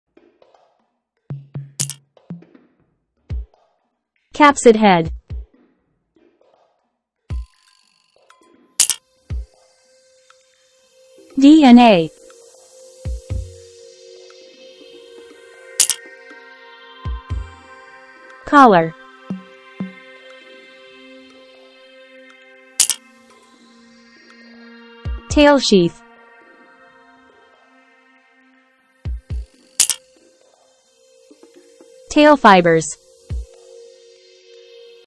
Bacteriophage Structure | Parts of a Bacterial Viruses
00:00 Introduction
00:04 Capsid head
00:11 DNA (genetic material)
00:18 Collar
00:25 Tail sheath
00:32 Tail fibers

Parts of a Bacteriophage
Bacteriophage Structure
Parts of a Bacterial Viruses